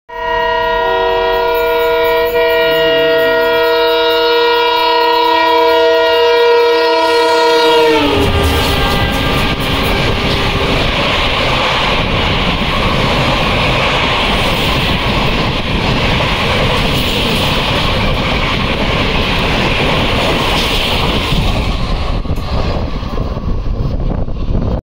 stromly crossing | रफ़्तार इतनी की दूसरी ट्रेन के डिब्बे हिलने लग गये😱😱
तूफानी गति से निकली बाड़मेर ऋषिकेश एक्सप्रेस